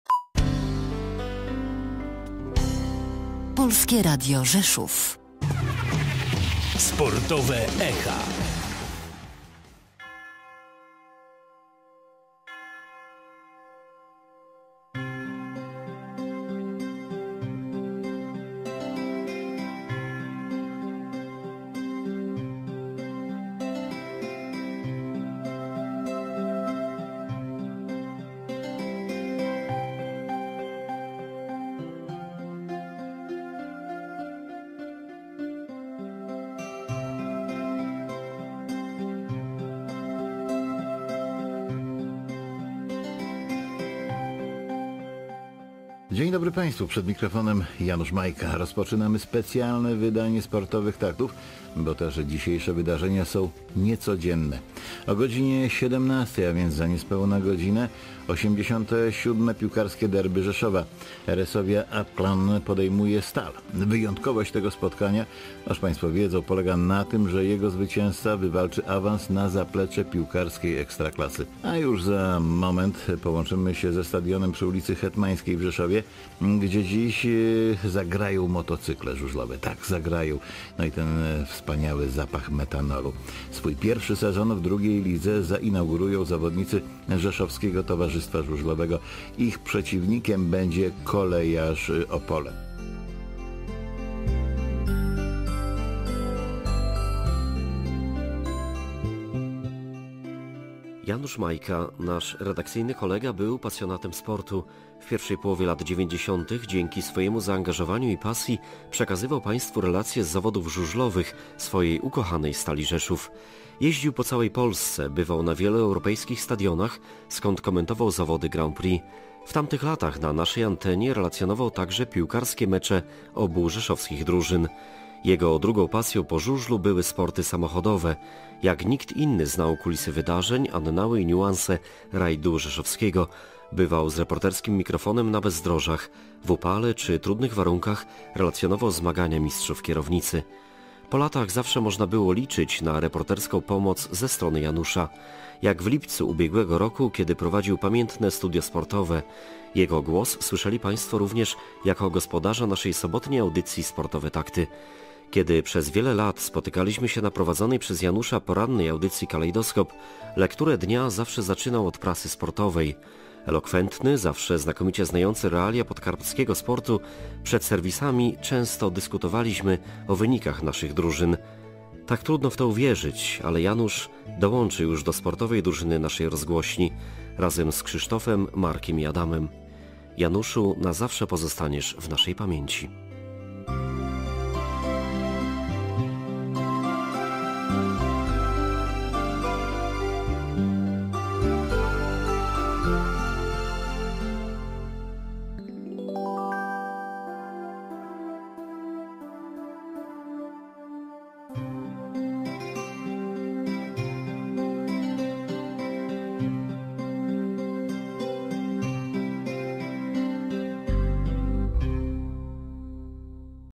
Wspomnienie redaktora Janusza Majki
Tak redaktora Janusza Majkę wspomina Bartłomiej Wisz – dziennikarz sportowy Polskiego Radia Rzeszów.

Janusz Majka był długoletnim dziennikarzem  Polskiego Radia Rzeszów. Zmarł 21 lutego 2021 r. w rzeszowskim szpitalu na oddziale covidowym z powodu niewydolności krążenia. Miał 64 lata.